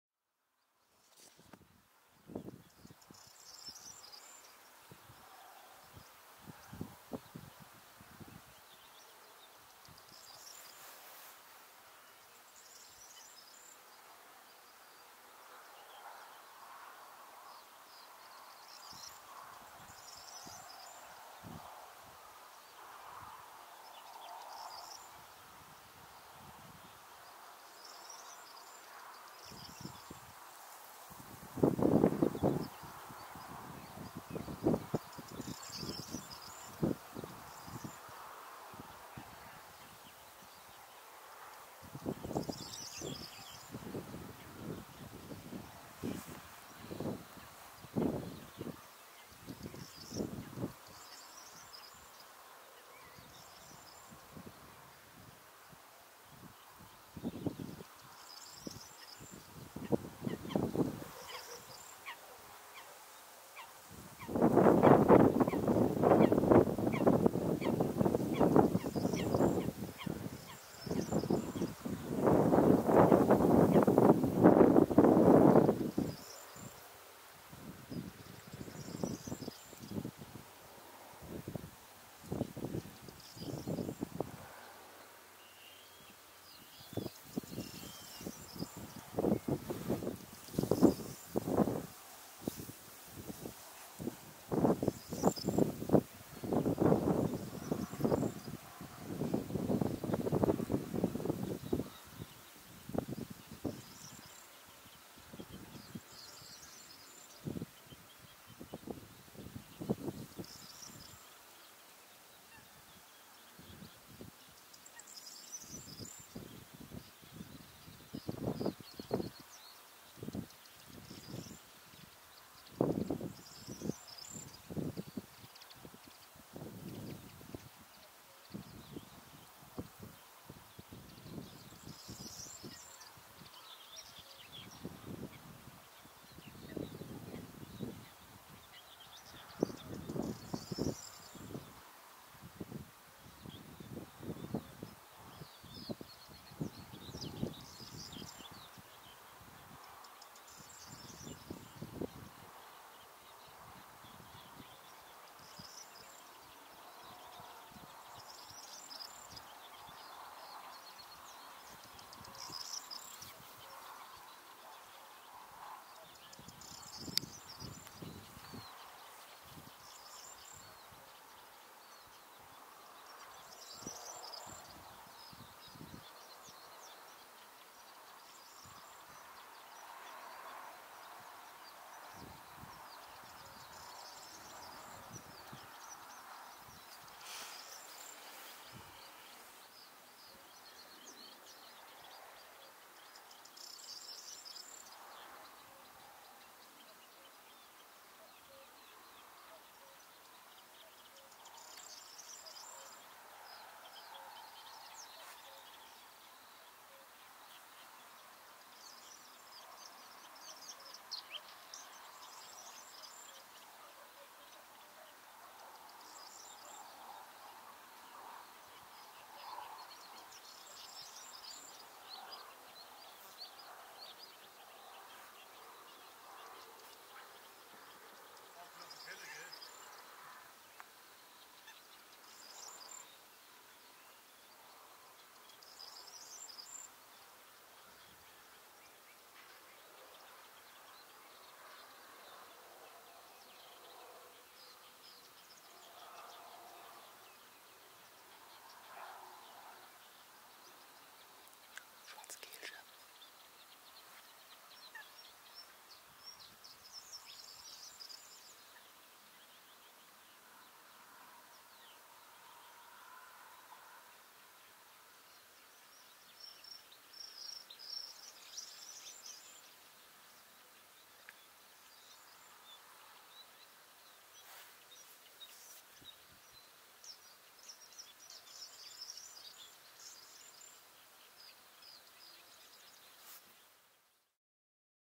Das Schwarzkehlchen
Heute am Sonntag konnte ich ein entspanntes Schwarzkehlchen filmen. Es putze sich keine 8 Meter vor mir.
Euer
Thomas